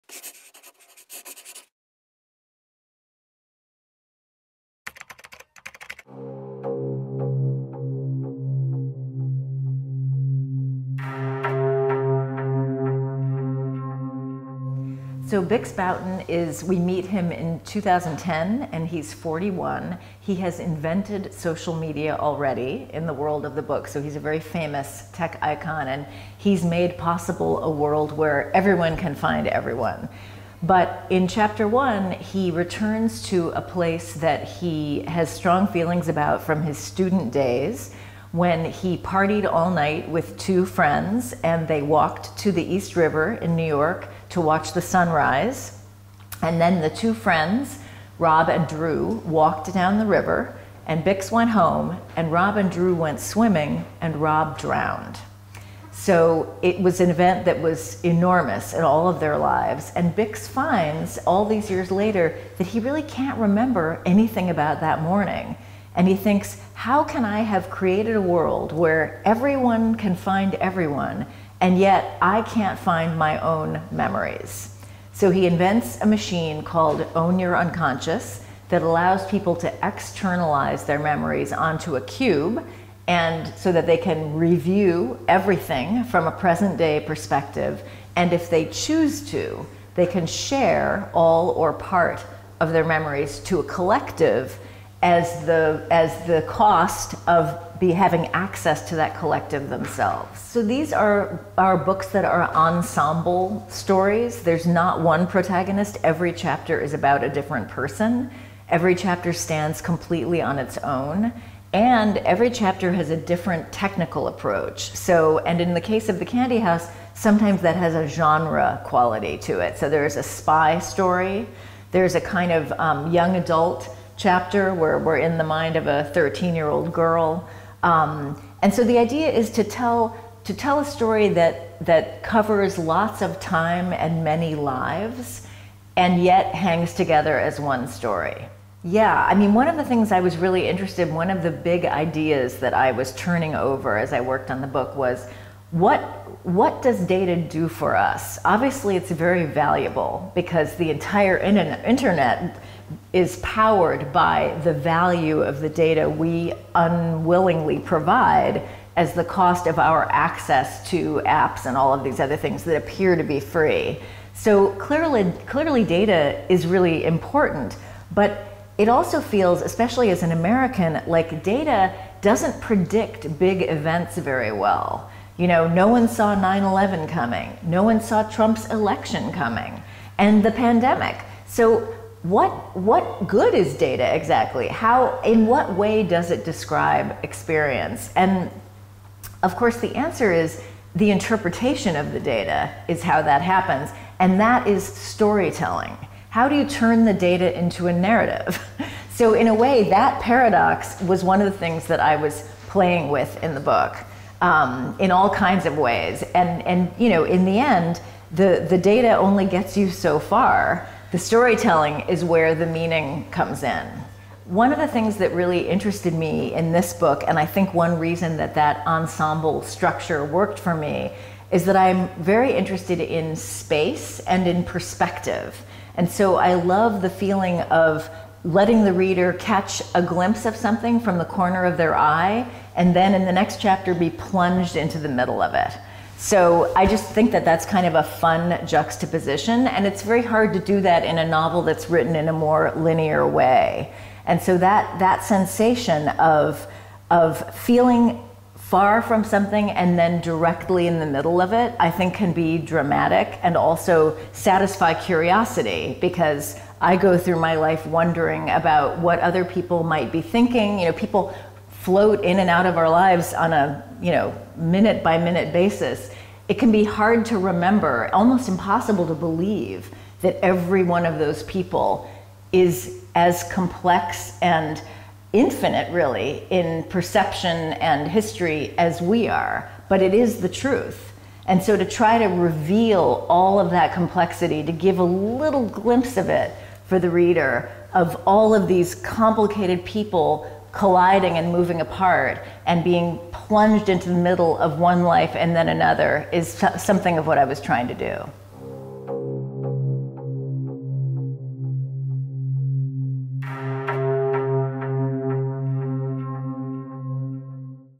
Jennifer Egan - La maison en pain d'épices
À l'occasion du festival "America" 2022, Jennifer Egan vous présente son ouvrage "La maison en pain d'épices" aux éditions R.Laffont.